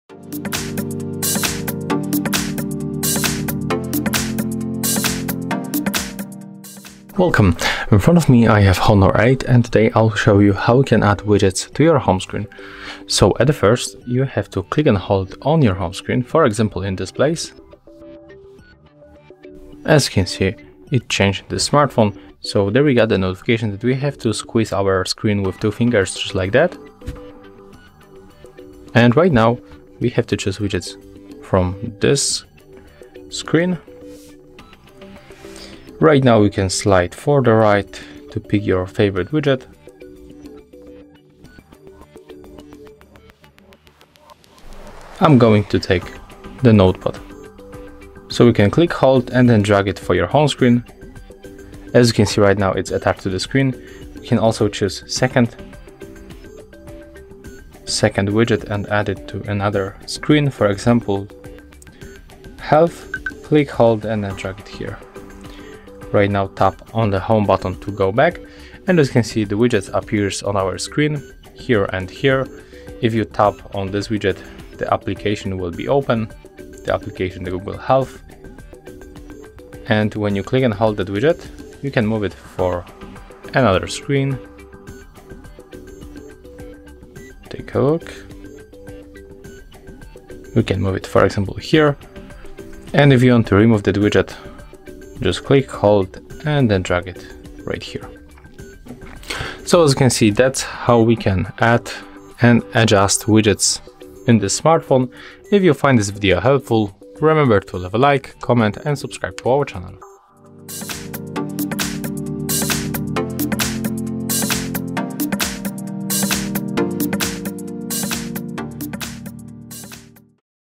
How to Add Widgets to Home Screen on Honor 8
Check more info about Honor 8:
Hi there! If you are wondering how to add more home screen functions in Honor 8, then we are going to show you! Do you know what Widgets are? Widgets are miniature application icons that are directly related to applications and are designed to make it easier for the user to access your favourite applications on your Honor 8 and are located on the home screen of the device. If you want to know how to add widgets to your home screen, then open the video attached at the top and our specialist will help you with this!

How to Add a New Option on the Home Screen on Honor 8?
How to Manage Screen Widgets on Honor 8?
How to Delete Widgets from Honor 8?